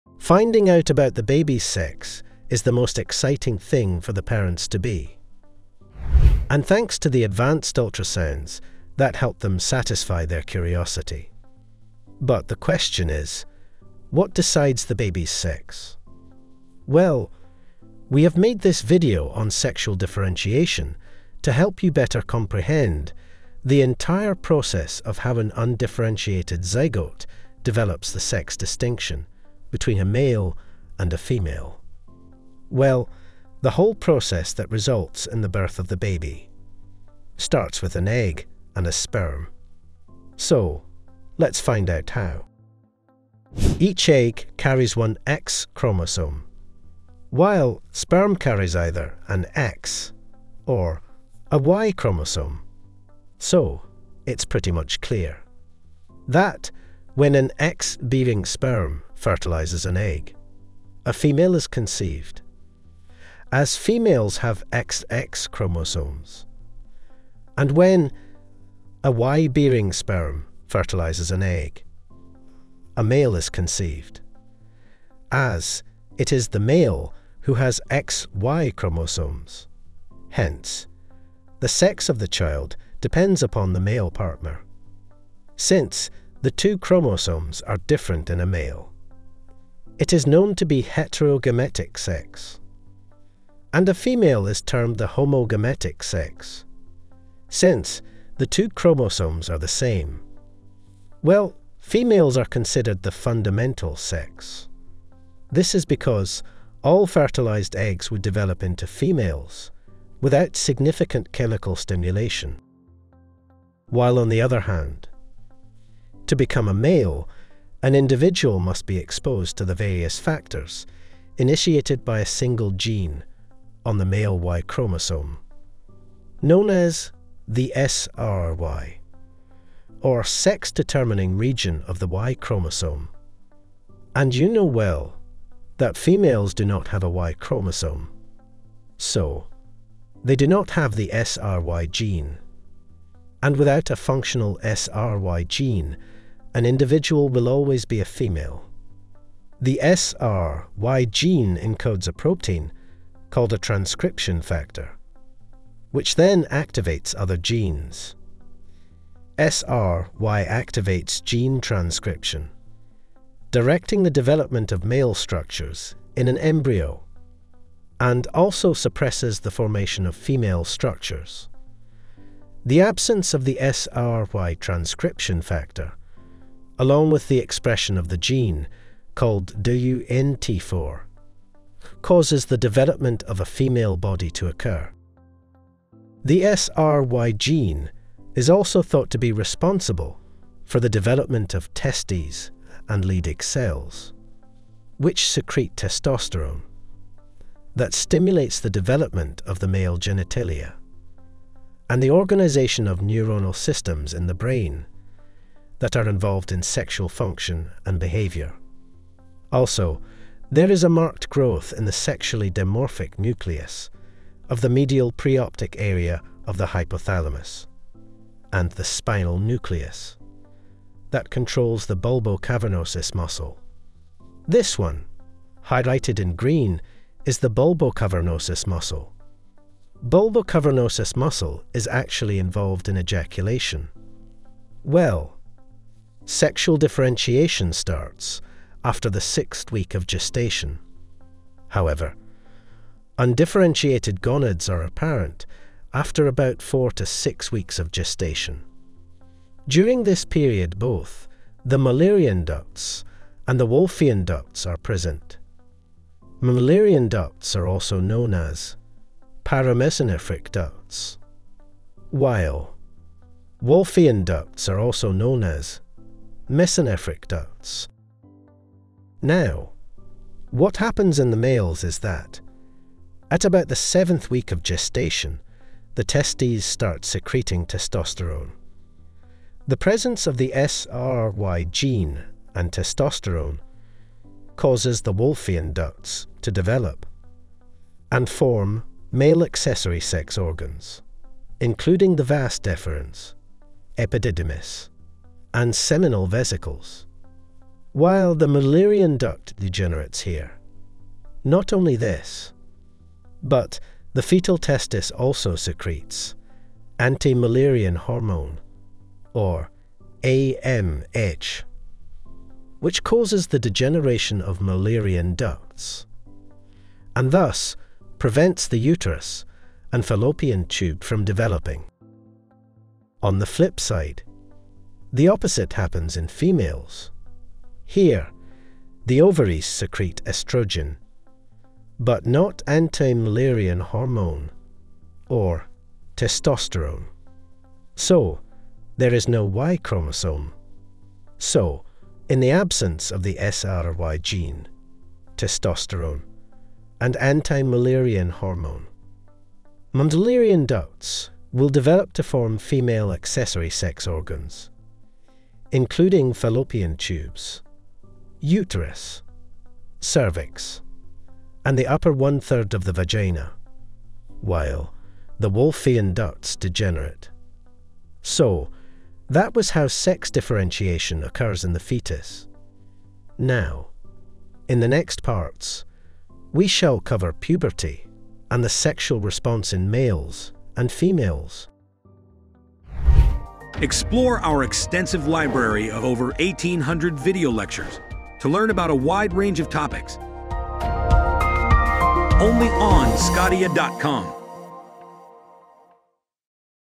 Sexual Differentiation and Function | Human Endocrine System Physiology
You can better understand the full process of how an undifferentiated zygote develops the ability to distinguish between sexes, changes that occur during puberty, and the sexual response cycle by watching this video on sexual differentiation.

▬  🕘 Timestamps
00:00 -  Sexual differentiation and function physiology
02:06 - Sex-determining Region of Y-Chromosome
06:23 - Link to video lectures on sqadia.com

▬ 📌 Sexual Differentiation
Undifferentiated gonads are apparent after about 4 to 6 weeks of gestation. During this period both the Müllerian ducts (paramesonephric ducts) and the Wolffian ducts (mesonephric ducts) are present. At about the 7th week of gestation:

In males - The testes start secreting testosterone and anti-Müllerian hormone (AMH). Their presence along with the SRY gene, causes the Wolffian ducts to develop and form male accessory sex organs, including the vas deferens, epididymis, and seminal vesicles. While the Müllerian duct degenerates here.

In females - Ovaries secrete estrogen, but not anti-müllerian hormone or testosterone. Müllerian ducts develop to form female accessory sex organs, including fallopian tubes, uterus, cervix, and the upper one-third of the vagina. While the Wolffian ducts degenerate.

▬ 📌 Physiology of Puberty
At puberty, the hypothalamus releases a hormone called gonadotropin-releasing hormone (GnRH).

Which then stimulates cells in the anterior pituitary called gonadotrophs, to secrete gonadotropins.

Gonadotropins include:

Follicle-stimulating hormone (FSH)
causes the release of Androgen-Binding Protein in males, and causes the maturation of follicles in females.

Luteinizing hormone (LH)
causes the secretion of the hormone, testosterone in males. While, it triggers ovulation and corpus luteum is formed, which causes the release of estrogen, progesterone, and inhibin.

The first sign of puberty in girls is the thelarche (breast development). This is followed by Pubarche (growth of pubic and axillary hair), and after that is menarche (first menstrual cycle).

An Important signal of sexual maturity in males includes spermarche (first ejaculation of semen).

You can learn about other changes at puberty, that occur in males and females, in the video.

▬ 📌 Sexual Response in Males and Females
The hypothalamus regulates overall libido under the influence of higher centers and the hormonal environment.

Well, in both males and females, the sexual response cycle comprises 4 phases. That is:
🔹Excitement
🔹Plateau
🔹Orgasm
🔹and Resolution

Though the phases are the same, the time and intensity of these phases, vary from person to person.

The video teaches you a lot more about these stages of the sexual response cycle in both males and females. So, do check it out!